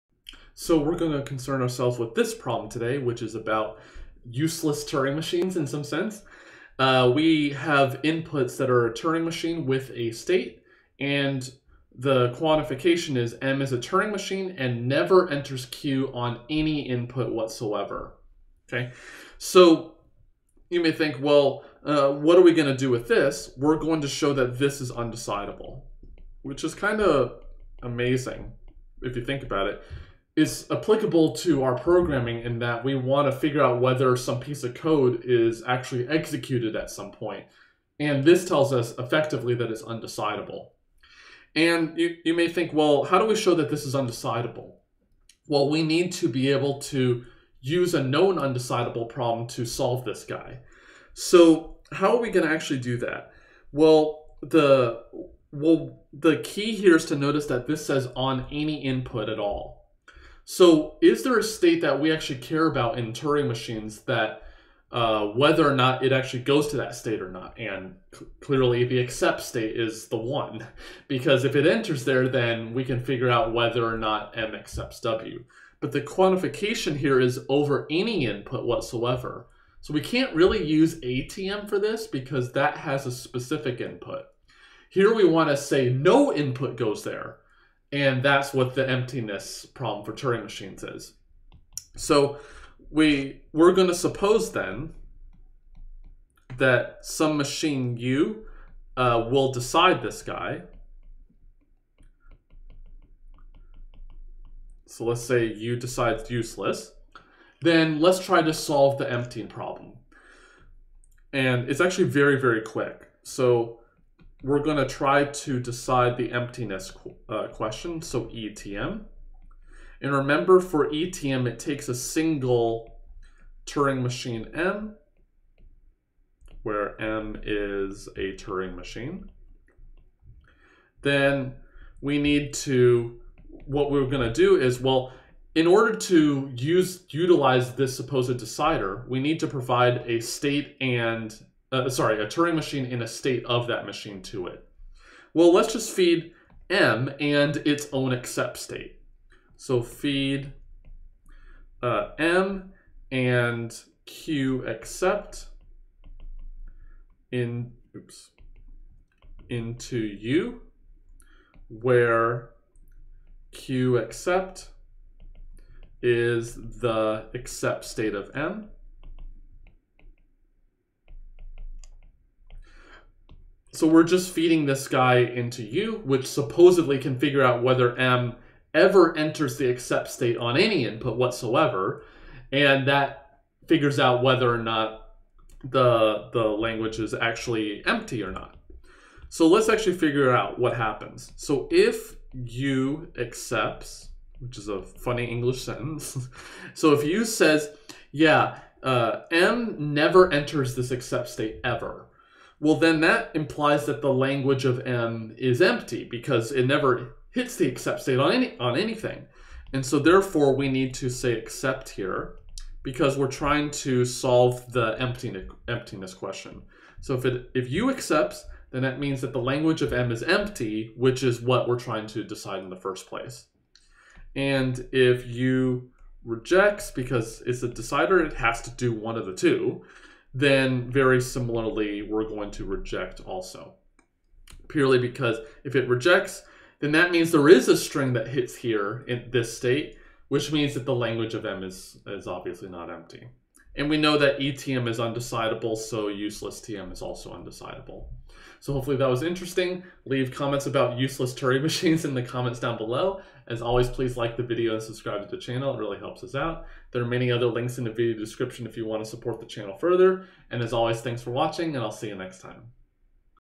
Useless Turing Machines are Undecidable
Here we show that determining if a Turing Machine state is "useless" is undecidable.

Gold Supporters: Micah Wood
Silver Supporters: Timmy Gy